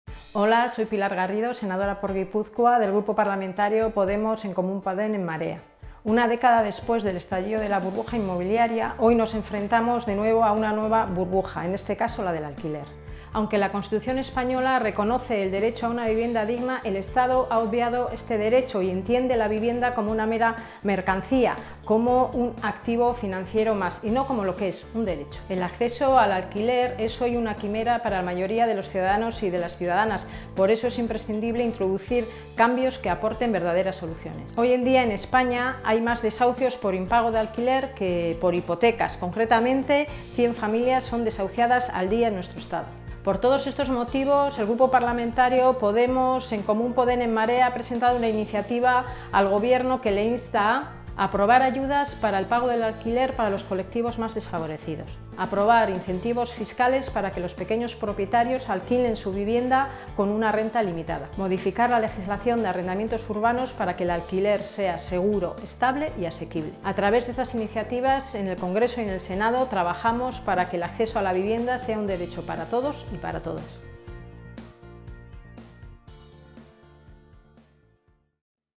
Pilar Garrido sobre la burbuja alquiler
Pilar Garrido nos explica que estamos viviendo un momento en el que la crisis se está trasladando de burbuja en burbuja.